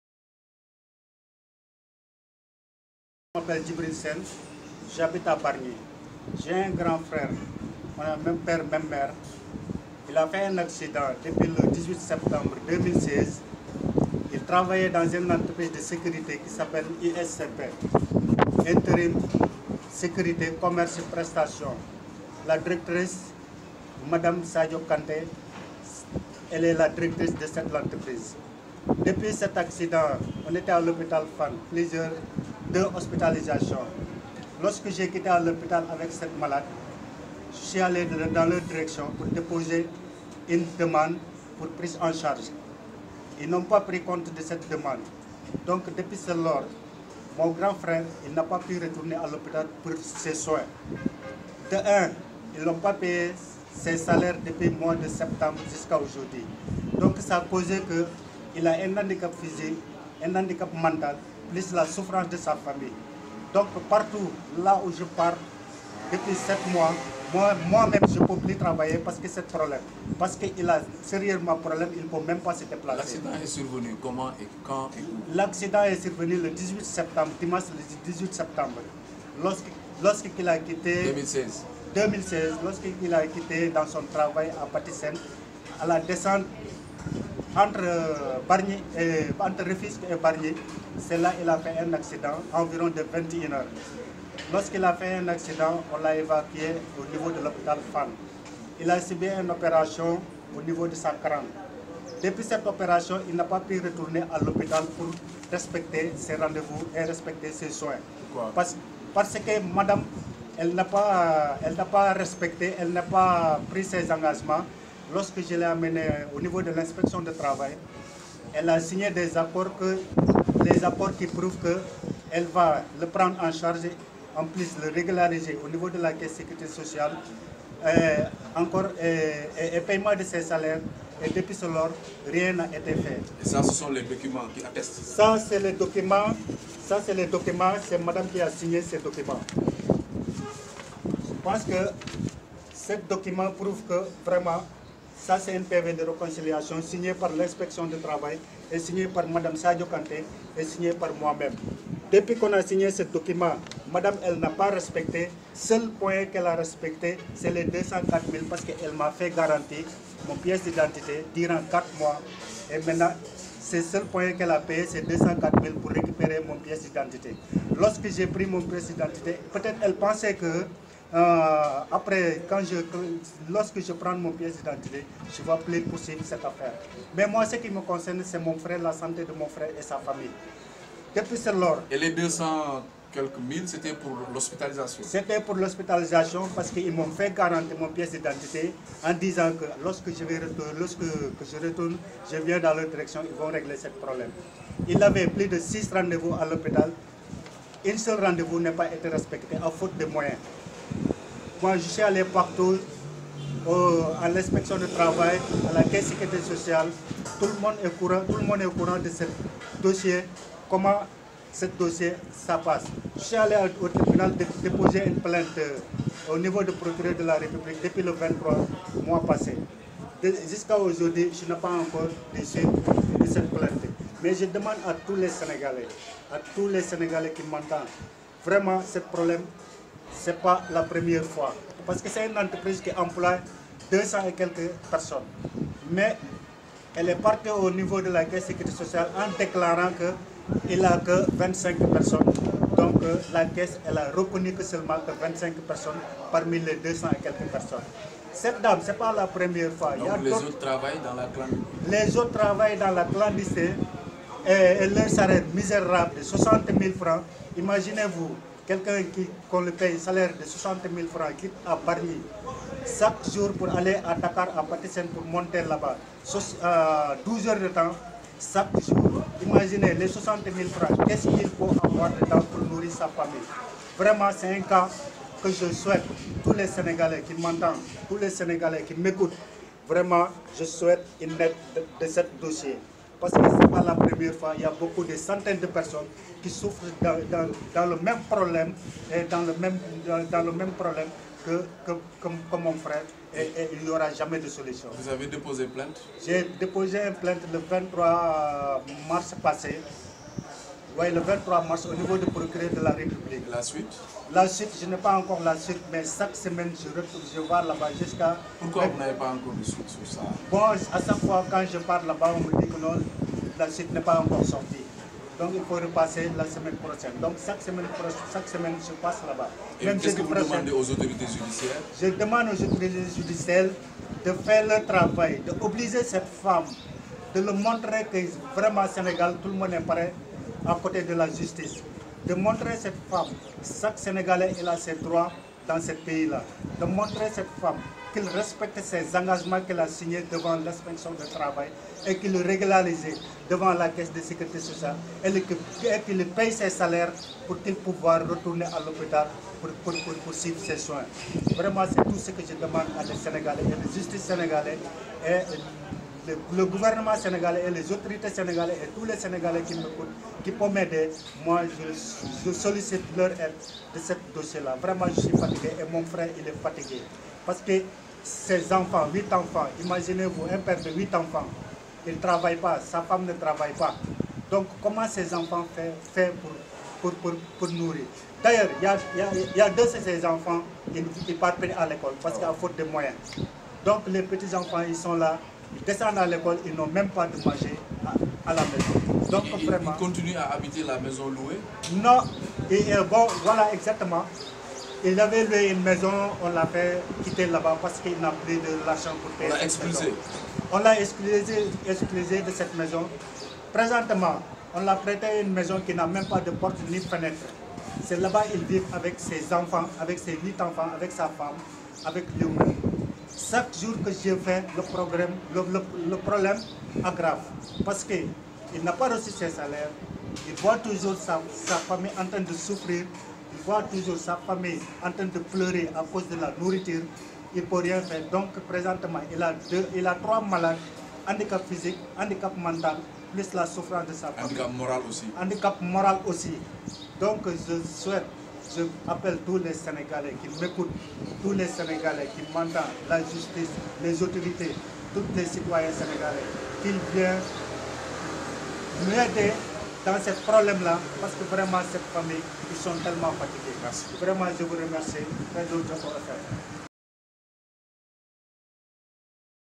Djibril Sene Raconte les malheurs de son grand frère qui a eu un accident
LVB TV
Djibril Sène jeune sénégalais résident à Bargny,  raconte les malheurs de son grand frère   qui a eu un accident sur le chemin de son lieu de travail. Depuis lors il a été infirme mentalement et a perdu ses moyens de travailler. Avec 8 enfants à nourir, l'homme n'arrive plus à même payer le loyer, il a été expulsé et ses enfants ne vons plus à l'école. C'est dur à entendre. Ames sensibles s'abstenir.